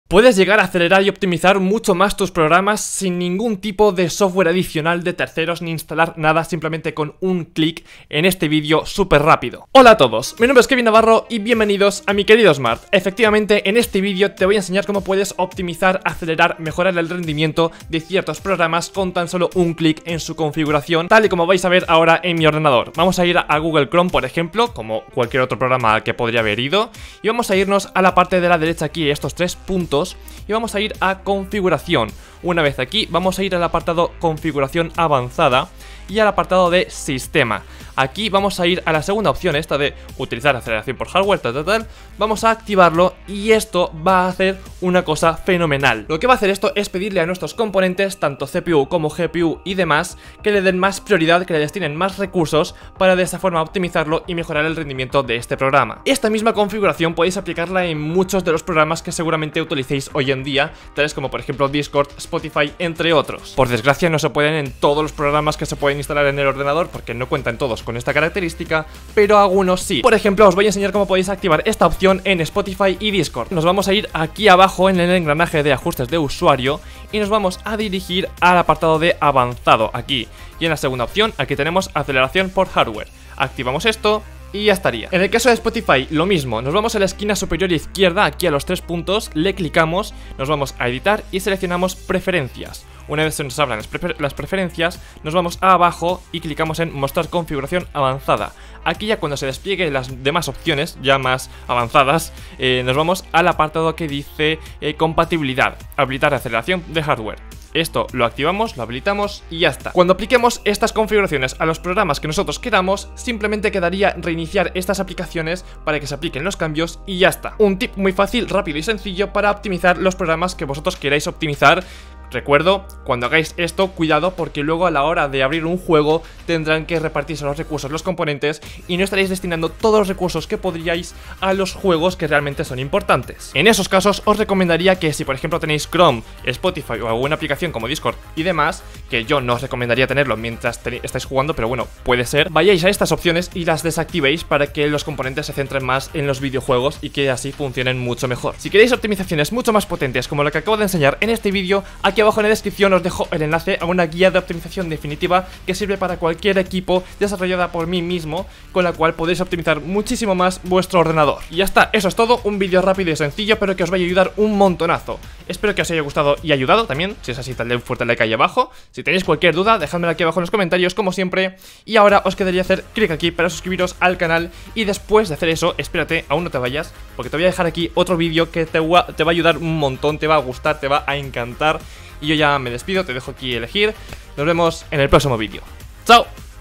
⚡ Cómo ACELERAR Google Chrome (y Cualquier Programa) EN 1 CLICK ✅ Guía de OPTIMIZACIÓN para PC
🚀 MEJORA el RENDIMIENTO de Cualquier Programa sin descargar aplicaciones con esta técnica llamada OPTICELERACIÓN ⬆️ Conseguirás acelerar el programa y mejorar su funcionamiento en Windows 10/11.
👇 Más OPTIMIZACIÓN para tu PC

📦 PRODUCTOS que Recomiendo 👇